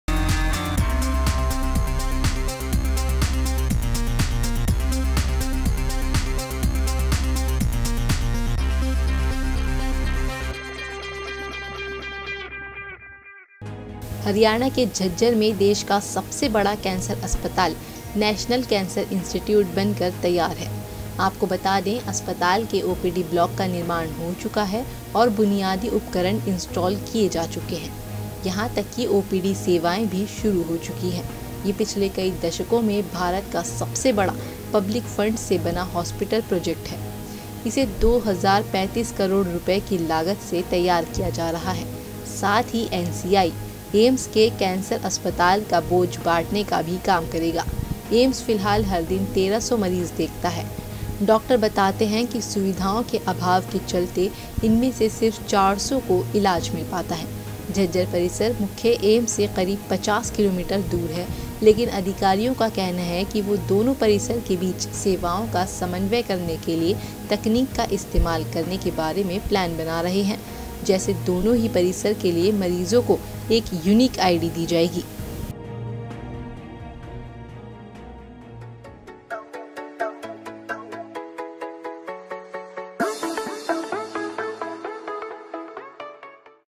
यहां देश का सबसे बड़ा कैंसर अस्पताल NCI बनकर तैयार !
हरियाणा के झज्जर में देश का सबसे बड़ा कैंसर अस्पताल नैशनल कैंसर इंस्टिट्यूट बनकर तैयार है। आपको बता दें अस्पताल के OPD ब्लॉक का निर्माण हो चुका है और बुनियादी उपकरण इंस्टॉल किए जा चुके हैं। यहां तक की OPD सेवाएं भी शुरू हो चुकी हैं।